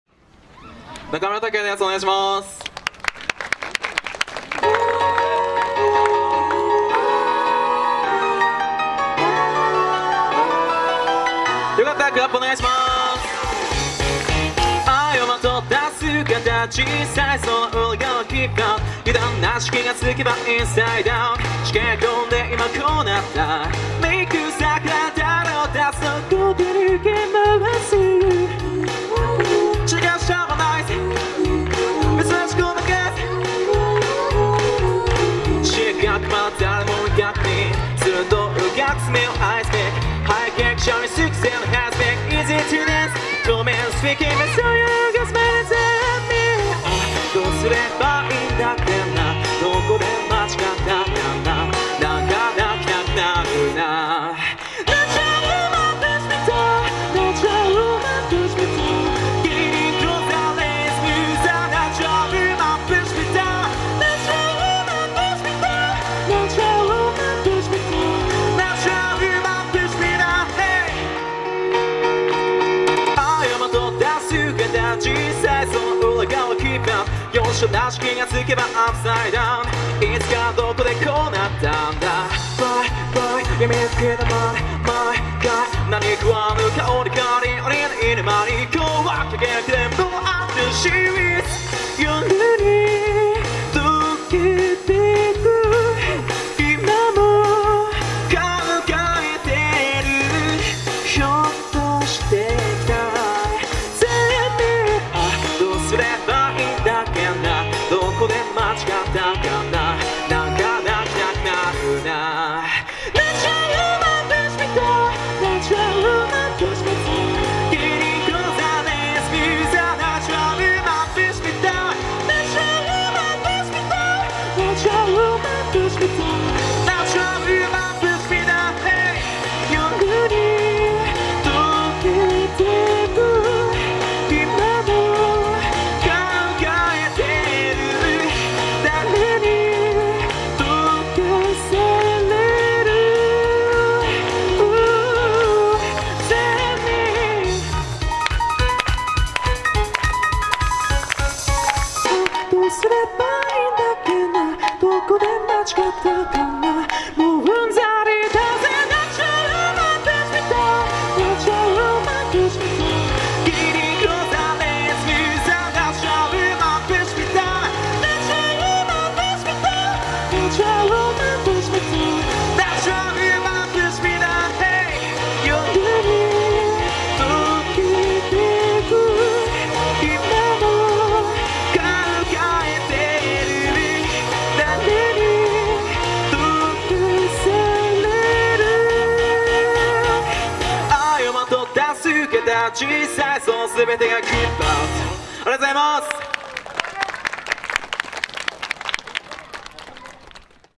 中村拓哉 ／ エイベックス・チャレンジステージ（名古屋城夏まつり）2018年8月10日
8月10日、名古屋城夏まつりでの「avex Challenge Stage（エイベックス・チャレンジステージ）」から、中村拓哉さんのステージ。